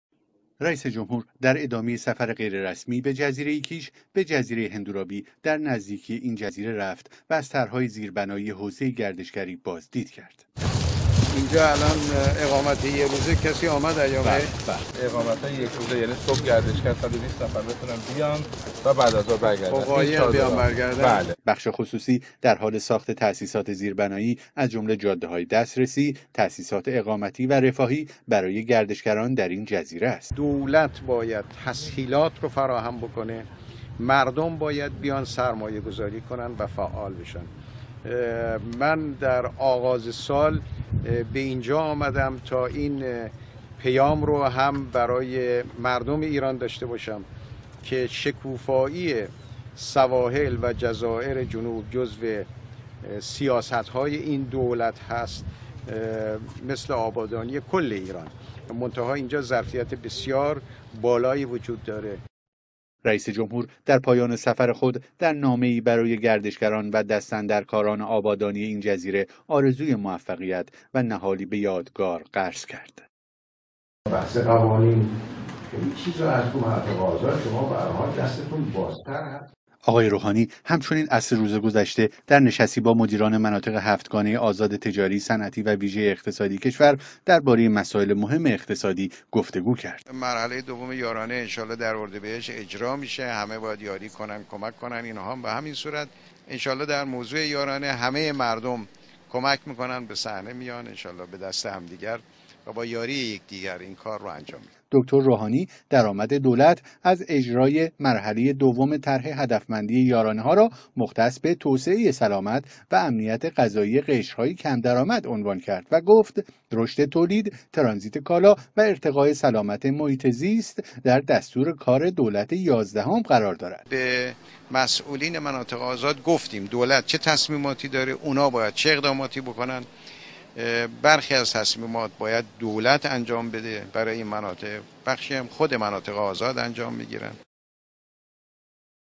Iran's President on Hendurabi Island, underlining the importance of promoting tourism. English sub.
Iran's President, Hassan Rouhani, visits Hendurabi Island near Kish and discusses the tourism industry and trade among other issues.